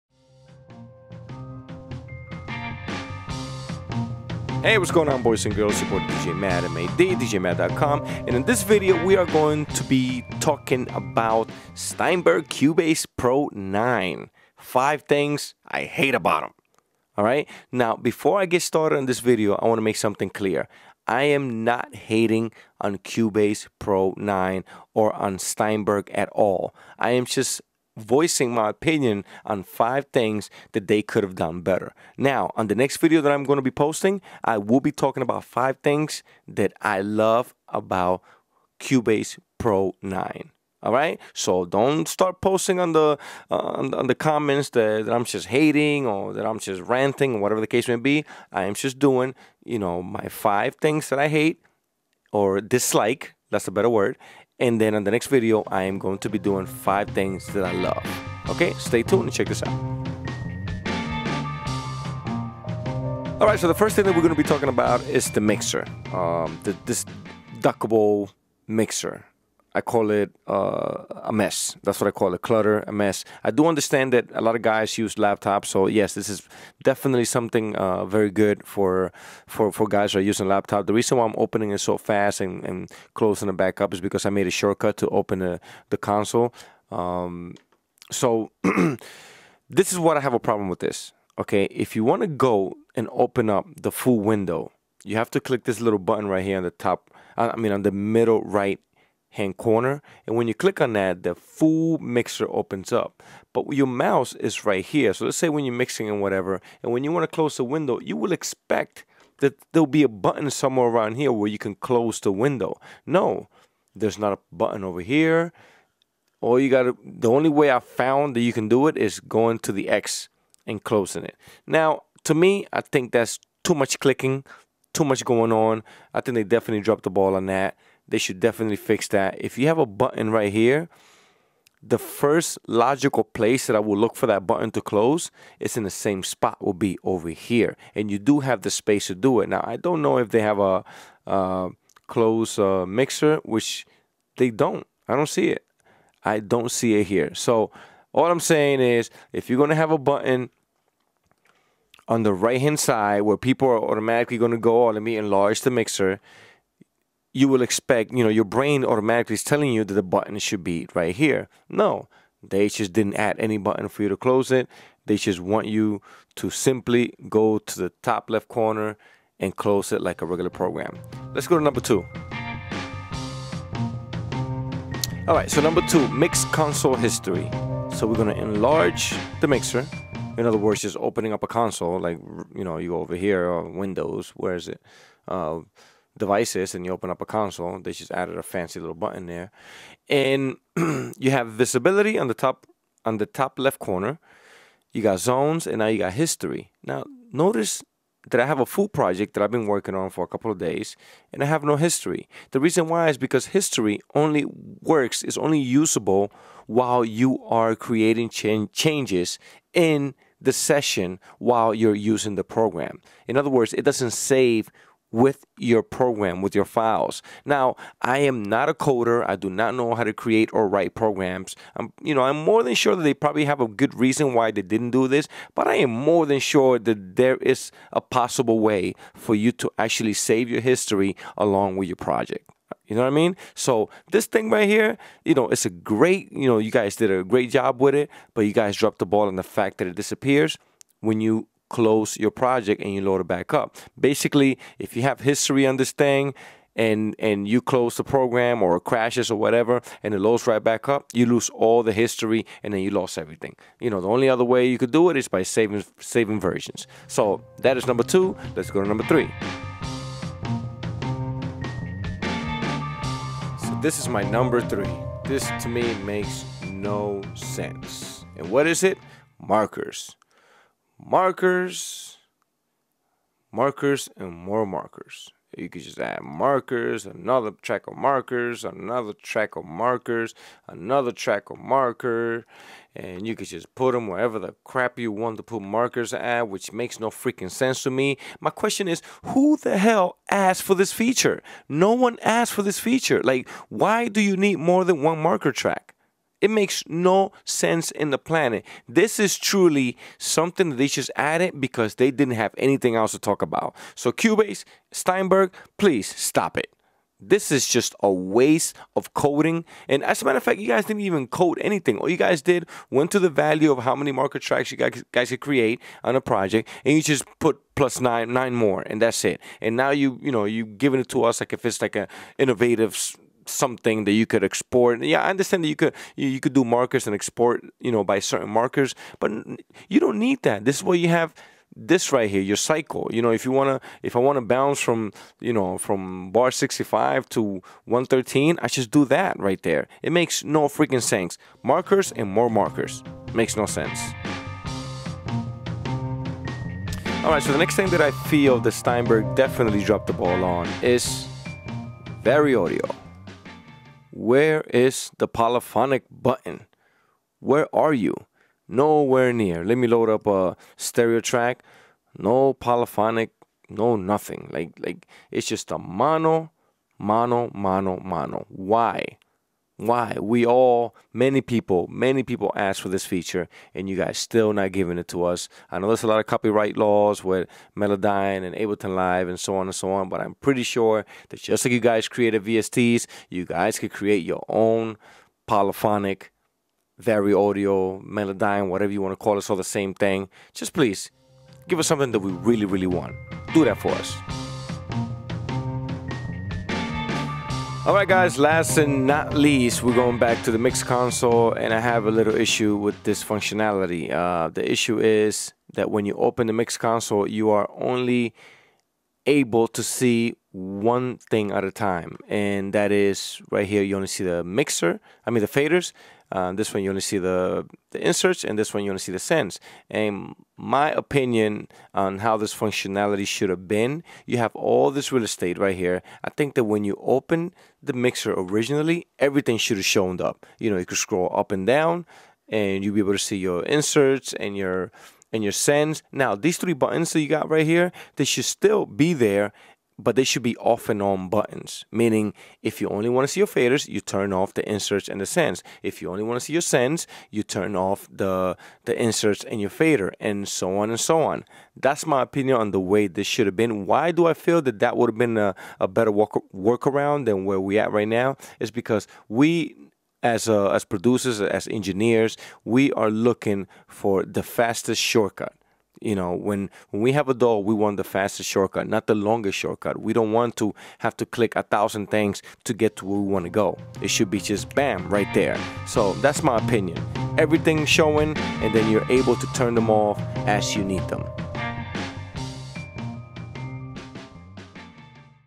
CUBASE Pro 9 - 5 Things I Dislike
What's going on guys, Here are my top 5 list of things i dislike about Cubase pro 9.